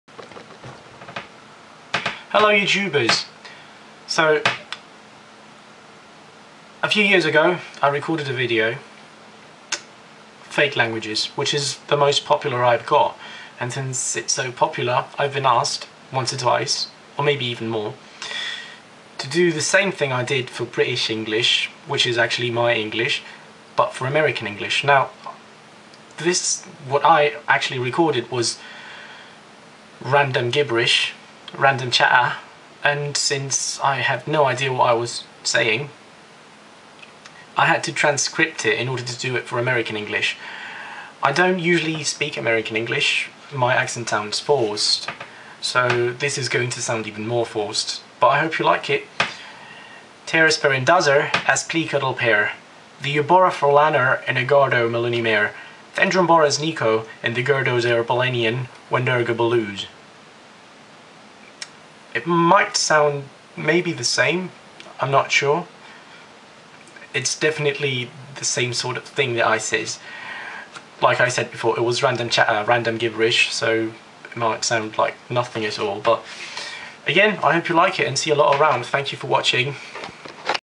Fake American English - ish
I spoke two types of Fake Spanish, it was only fair to speak two types of Fake English...